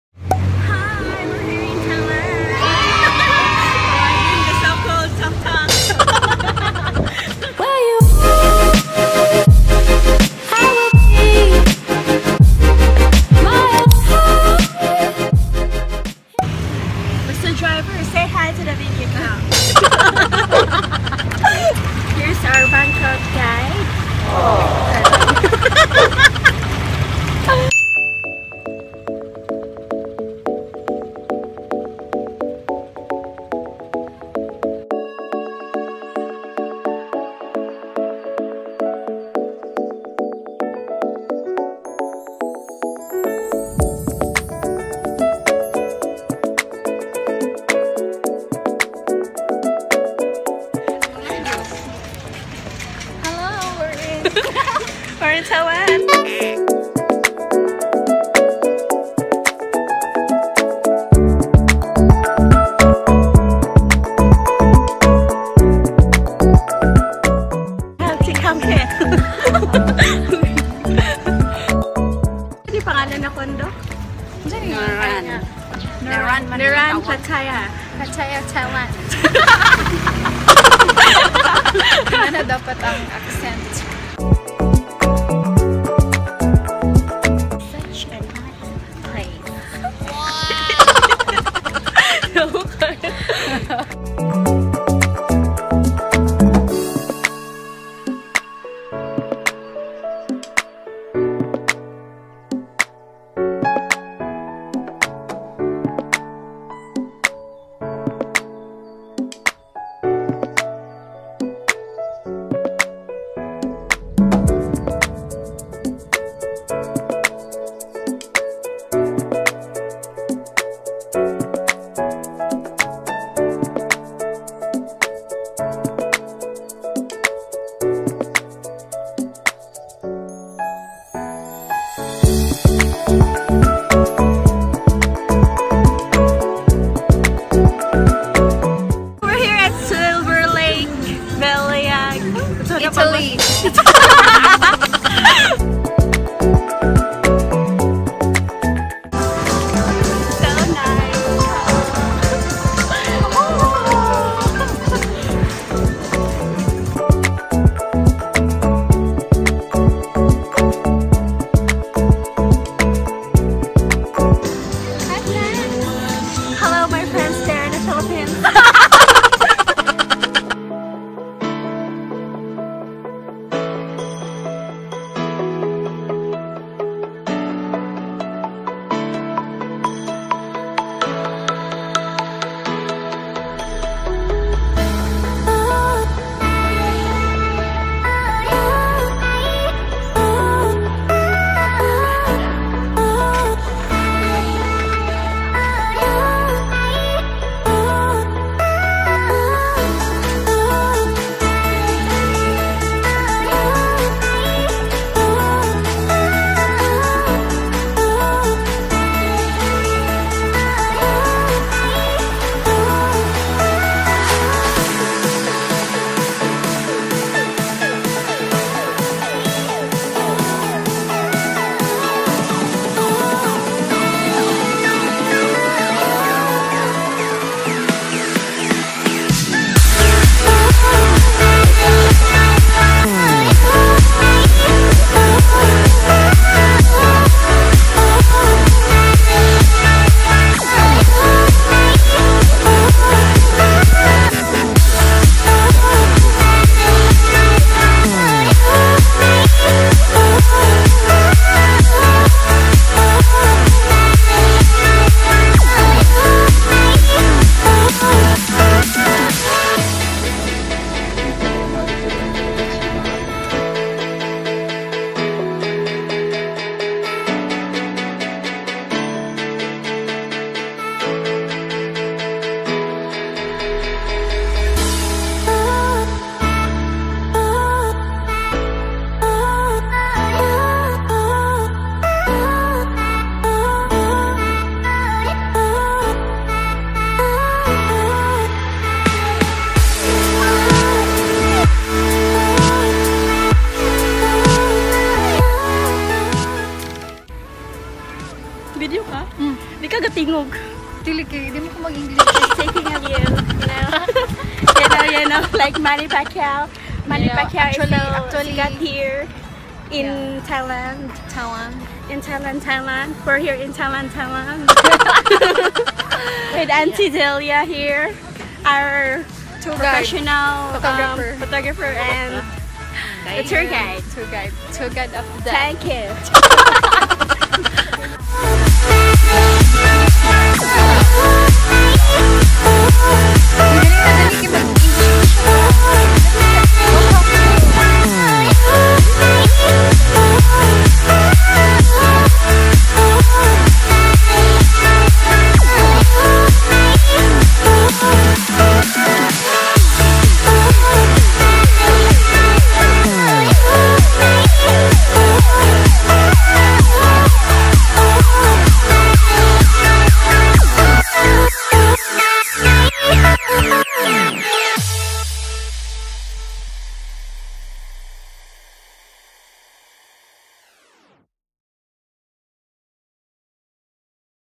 SILVERLAKE PATTAYA THAILAND - THAILAND TRAVEL VLOG pt.1 ❤ | Ann Cinco
If you're sensitive to throwback videos, please skip this one :)

My bestfriend and I had a day trip to Pattaya from Bangkok, rode a bus from our hotel, and met my Aunt Delia and other relatives there.

They toured us around the city, though one day is not enough to fully enjoy the place.

Planning to go back with the whole fam soon!